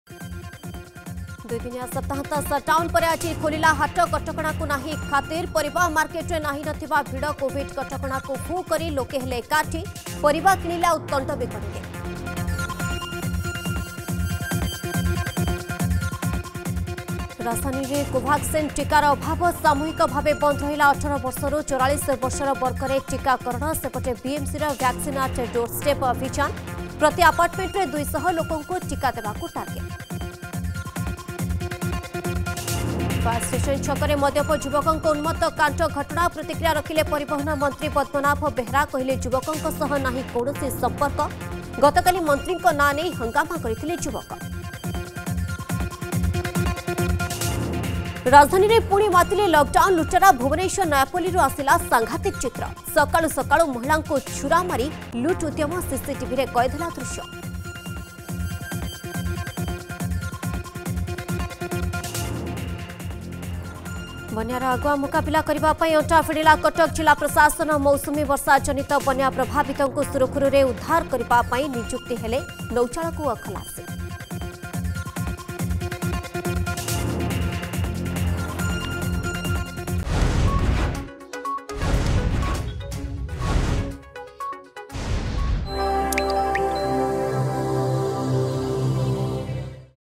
Headlines 6 PM | 31 May 2021 | Argus News
+ 2 ଦିନିଆ ସପ୍ତାହନ୍ତ ସଟଡାଉନ ପରେ ଆଜି ଖୋଲିଲା ହାଟ
+ ରାଜଧାନୀରେ କୋଭାକ୍ସିନ୍ ଟିକାର ଅଭାବ
+ ଫାୟାର ଷ୍ଟେସନ ଛକରେ ମଦ୍ୟପ ଯୁବକଙ୍କ ଉନ୍ମତ କାଣ୍ଡ ଘଟଣା
+ ରାଜଧାନୀରେ ପୁଣି ମାତିଲେ ଲକଡାଉନ ଲୁଟେରା
+ ବନ୍ୟାର ଆଗୁଆ ମୁକାବିଲା କରିବା ପାଇଁ ଅଣ୍ଟା ଭିଡ଼ିଲା କଟକ ଜିଲ୍ଲା ପ୍ରଶାସନ